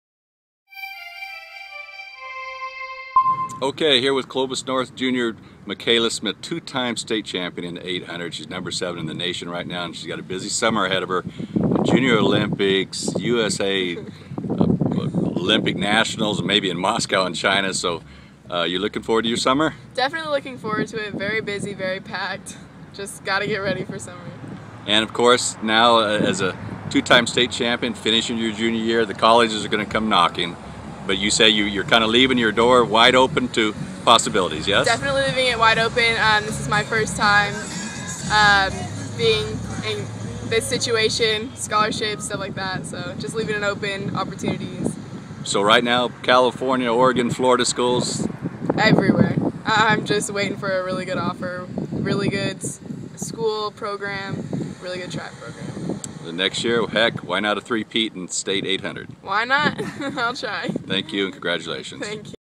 Clovis North track star Mikaela Smith emerging as national recruit
Clovis North's Mikaela Smith, two-time California champion at 800 meters, is only a junior. Video: Andy Boogaard.